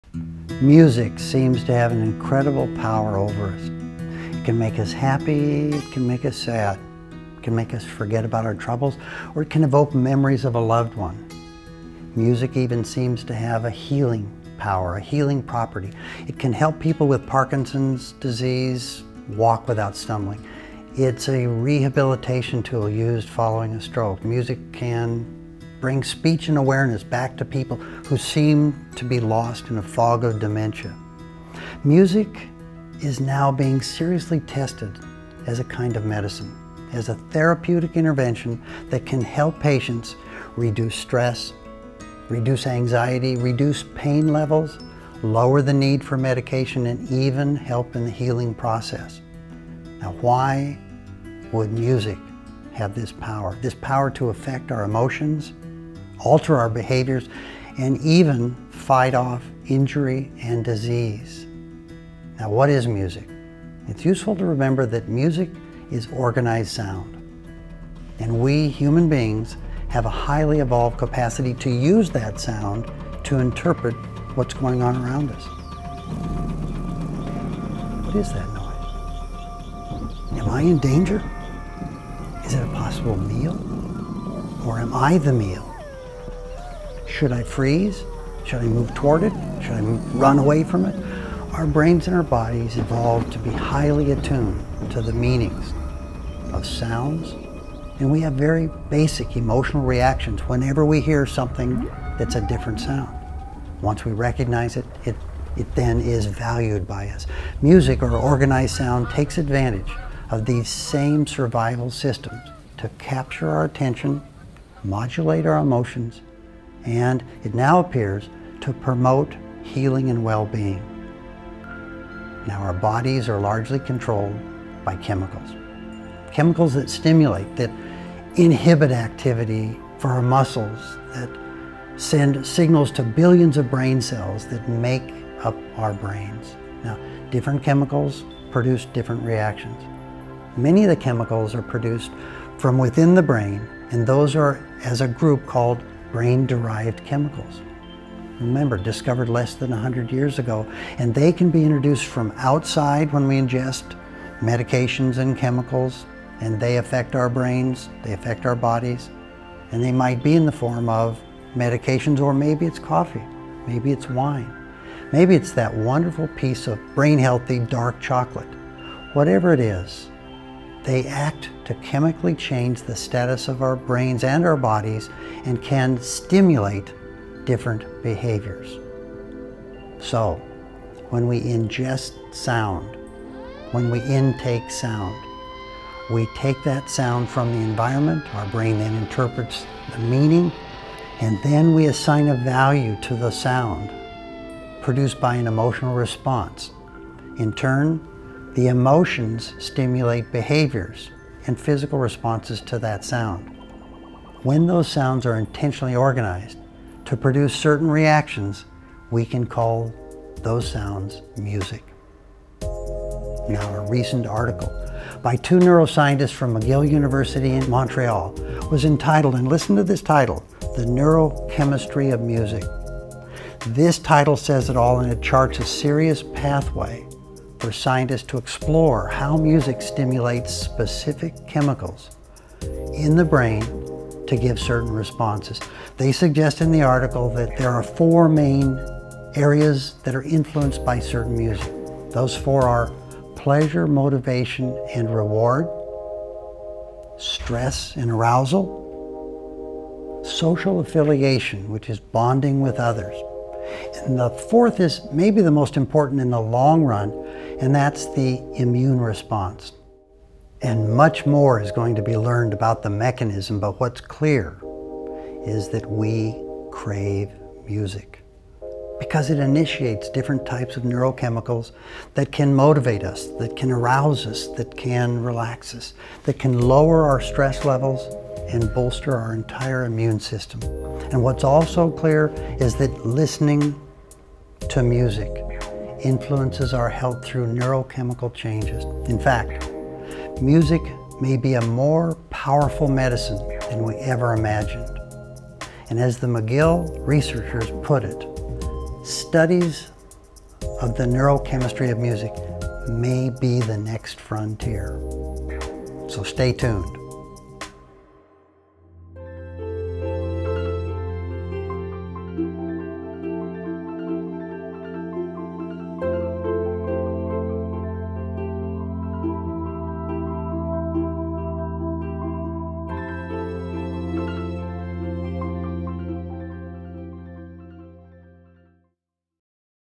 Roger Anunsen "The Aging Mind: The Power of Music"
This is video 4 in Roger Anunsen's "The Aging Mind" video series.

Roger Anunsen is a Gerontology faculty member at Portland Community College in Portland, Oregon.

This video was produced in collaboration between the Gerontology Program, Distance Education and the Video Production Unit at PCC.

Written by Roger Anunsen & Michael C. Patterson
Performed by Roger Anunsen
Producers: Michael Annus & Monica Martinez-Gallagher
Director: Michael Annus
Camera: Kevin Forrest
Grip/teleprompter: Brad Norton
Lighting: Mike McNamara
Editor: Lucia DeLisa

Image and music credits:
Chanda, ML and Levitin, DJ, The Neurochemistry of Music,
Trends in Cognitive Sciences, 2013 Apr; 17(4):179-93.
Animal sound effects:
      US Fish and Wildlife Service via Wikimedia Commons
Thunder sounds:
      Richard Humphries via Wikimedia Commons
Music used under license from Audio Network

Photos:
Sheet music, Guitar, Trombone player: Lucia DeLisa
Coffee cup: Dennis Gnad (via WikiMedia Commons)
Wine glass: Fiona Shields (via WikiMedia Commons)
Chocolate: MZissou (via WikiMedia Commons)
All other photos are public domain (via WikiMedia Commons)